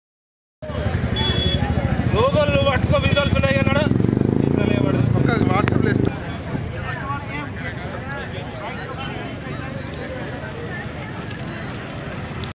ట్రాక్టర్ ఢీకొని టూవీలర్ దగ్దం - 29-11-2017
కరీంనగర్ సప్తగిరి కాలనీ బైపాస్ రోడ్డుపై టూవీలరక్ ట్రాక్టర్ ఢీకొన్న ఘటనలో టూవీలర్ దగ్దం. సంతోష్ అనే వ్యక్తికి తీవ్రగాయాలు